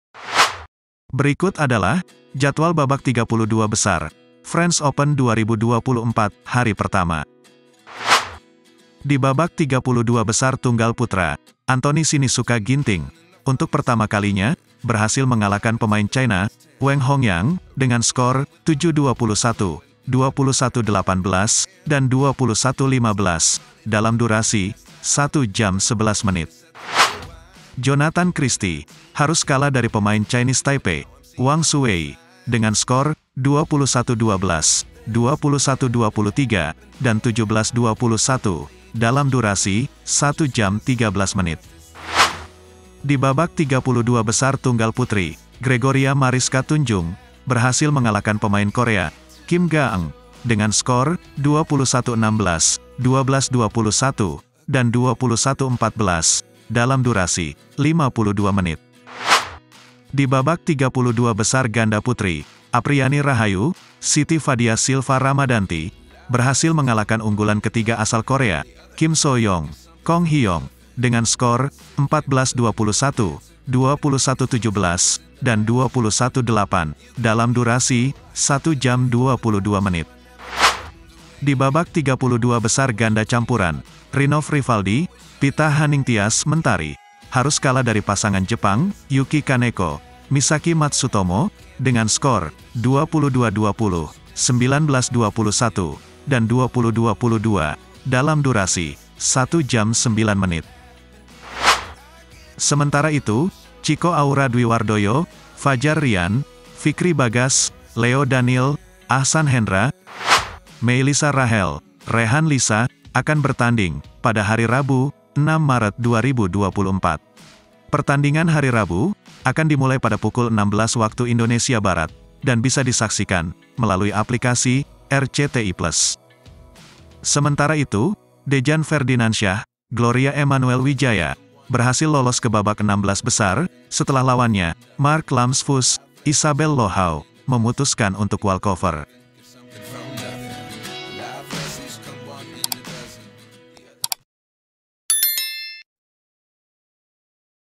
Hasil French Open 2024 Day 1 Babak 32 Besar Tadi Malam. Tiga Menang & Dua Kalah
Hasil Lengkap French Open 2024 Day 1 Babak 32 Besar Tadi Malam. Anthony Sinisuka Ginting, Gregoria Marika Tunjung, Apriyani Rahayu/Siti Fadia Silva Ramadhanti berhasil lolos ke babak 16 besar.